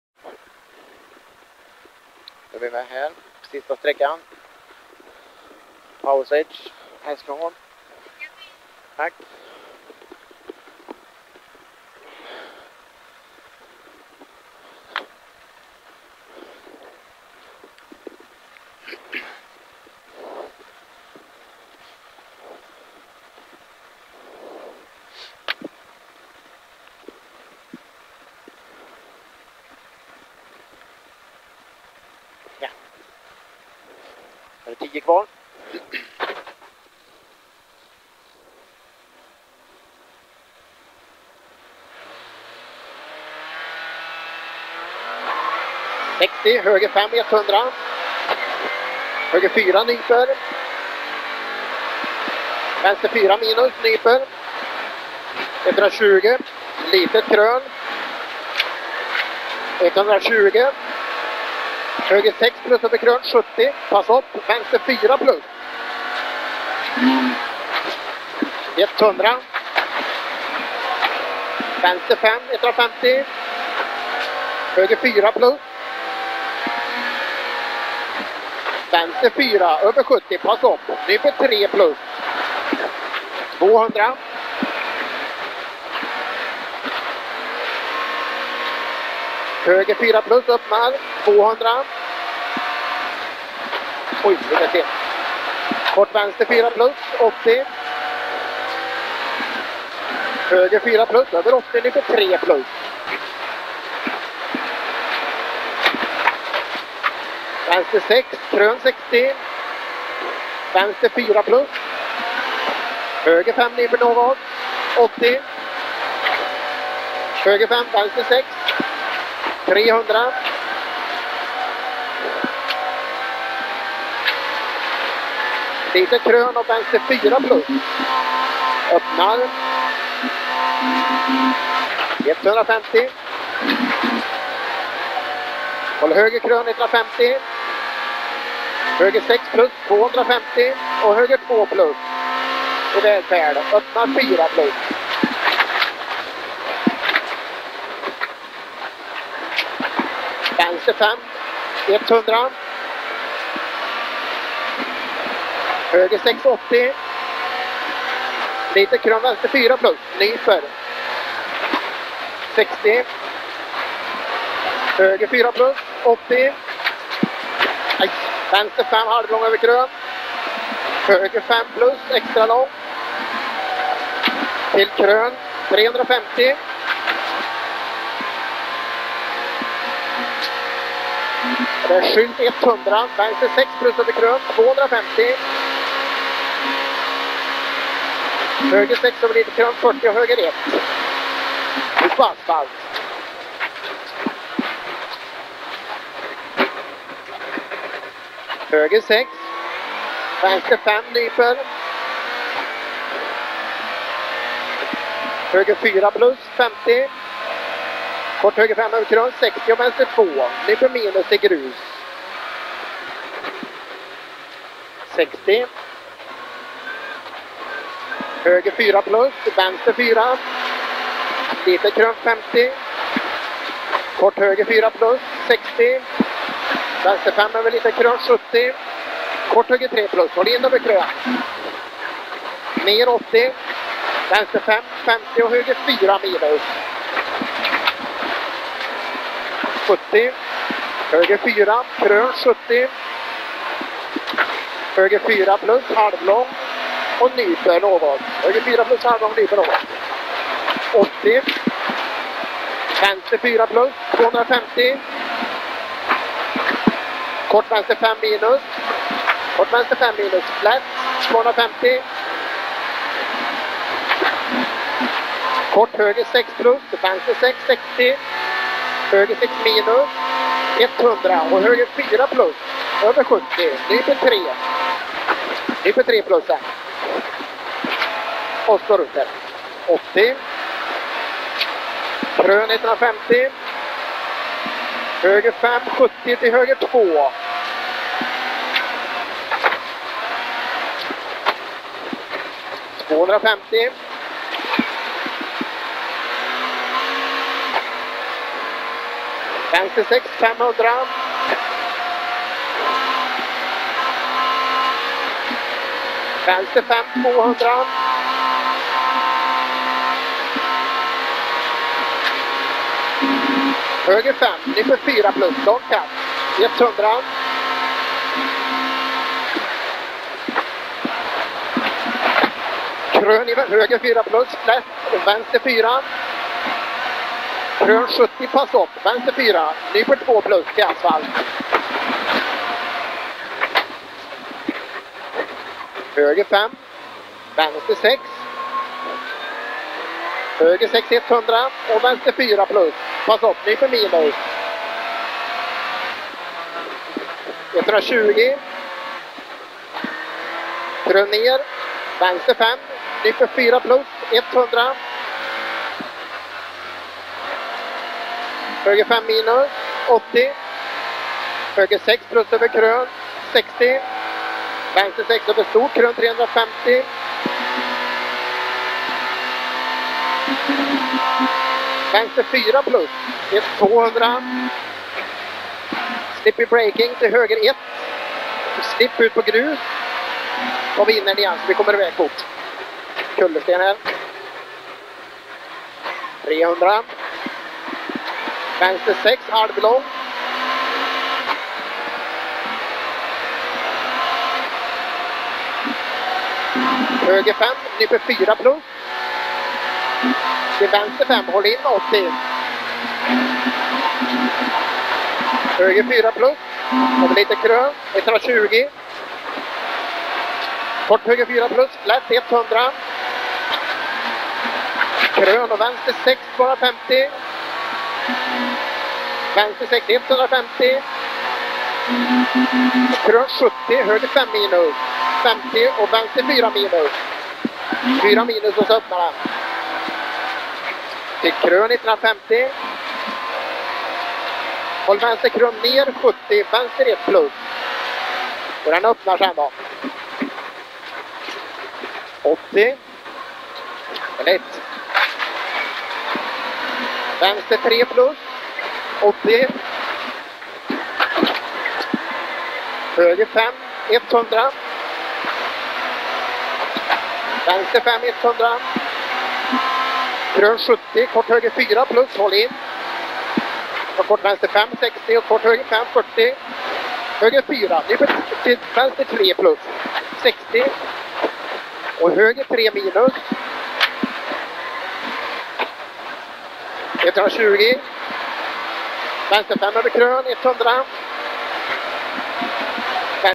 Flodin Bergsten BMW M3 Snapphane 2018 ss8 powerstage ej sista km
Avgörande ss rally-sm Snapphane, låg 12 sek efter ledning men tar här igen 15 sek och vinner tävlingen. Tyvärr en inställning på kameran som gör det lite svajigt och att sista minuten inte finns med!